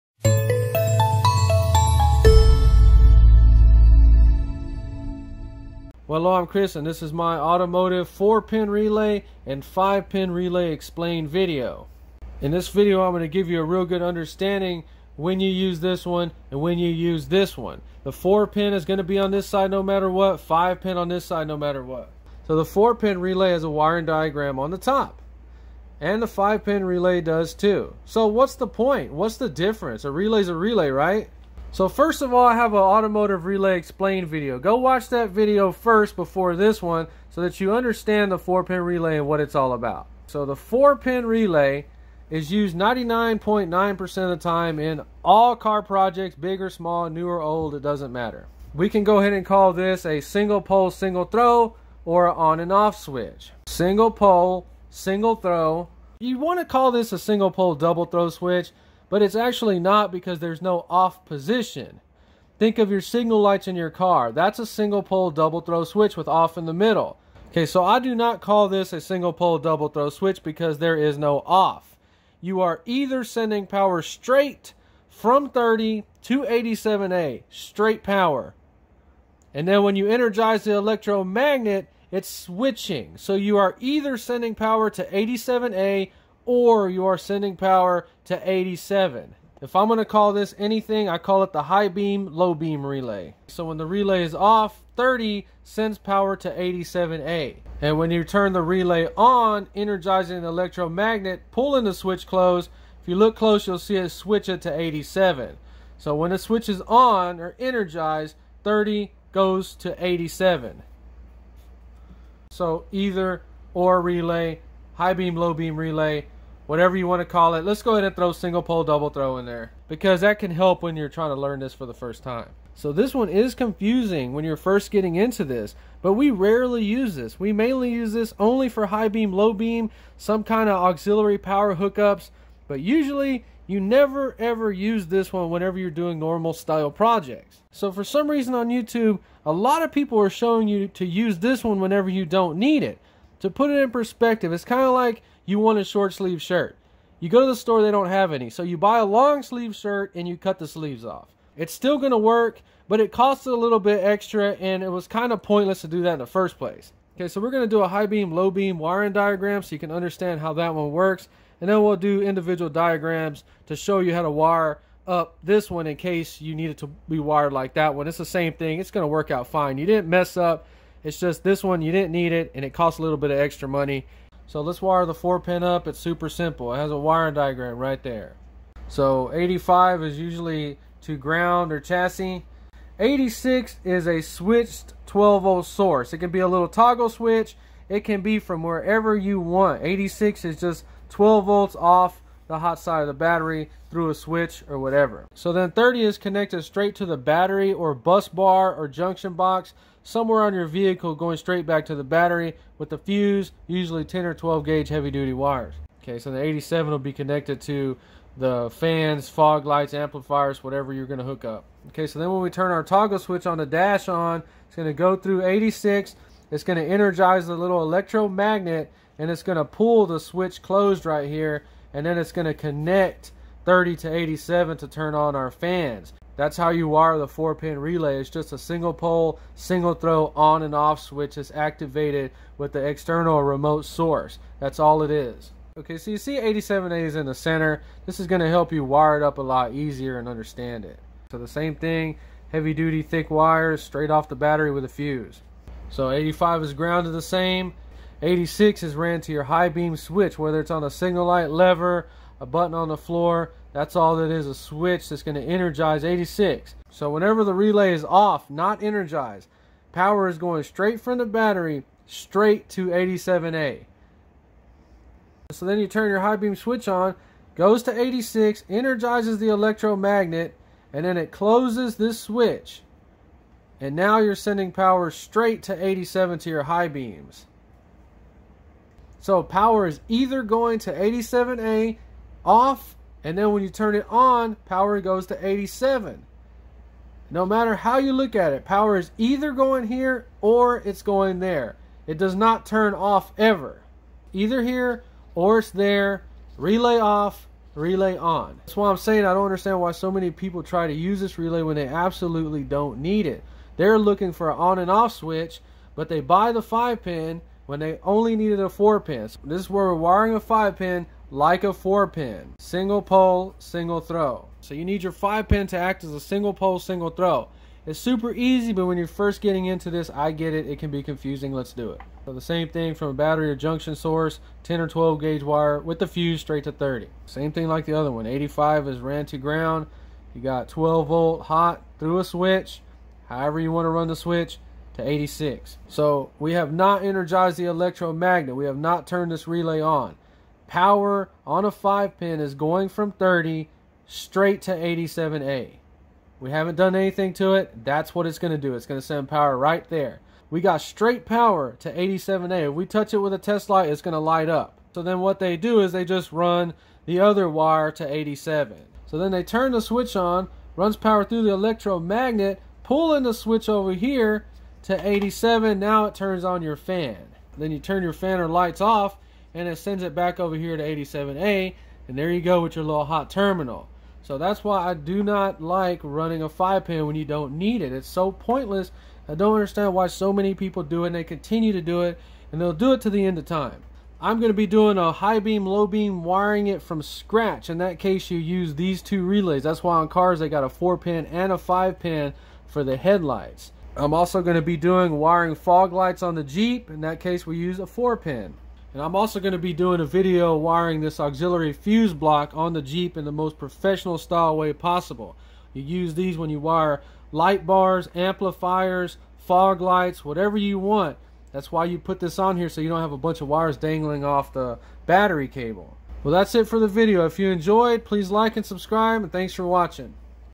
automotive 4 pin and 5 pin RELAY EXPLAINED which one?
just an idea on how to wire each relay up...